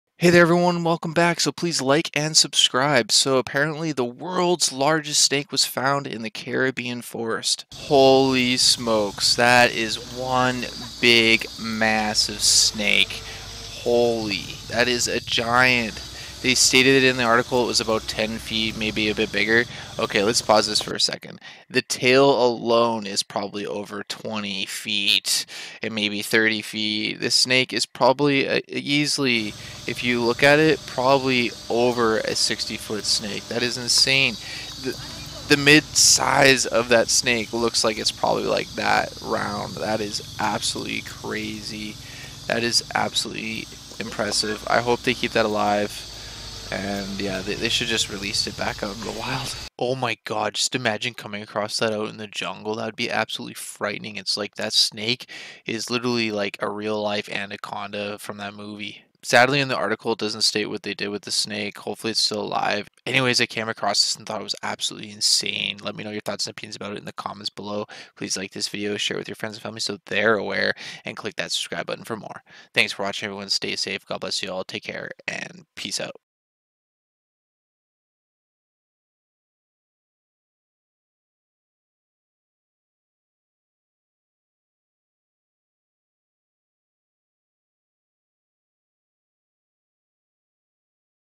World's Biggest Snake Found In Caribbean Forest
Jaw-dropping footage shows a “world record” snake being lifted up by a digger.
The huge beast appears to have been found by workers clearing part of the Dominica rainforest.
In the footage, the astonished man filming can be heard saying: “My mother, what?”
The snake, which is at least 10-feet long, is alive as it’s lifted by the digger.
Since it was uploaded to social media last week, the video has been viewed millions of times.
Reddit users expressed their amazement at the size of the beast.
One said: “That looks like some world record s**t.”
Another posted: “For real. That thing’s actually jumbo.”
A third wrote: “Snakes on a crane.”
It is not clear what kind of snake appears in the video.
Dominica, a Caribbean paradise measuring just 29 miles long and 16 miles wide, has been dubbed “the Nature Island” thanks to its array of wildlife.
One species of snake native to the island is the deadly boa constrictor which can grow up to 13 feet.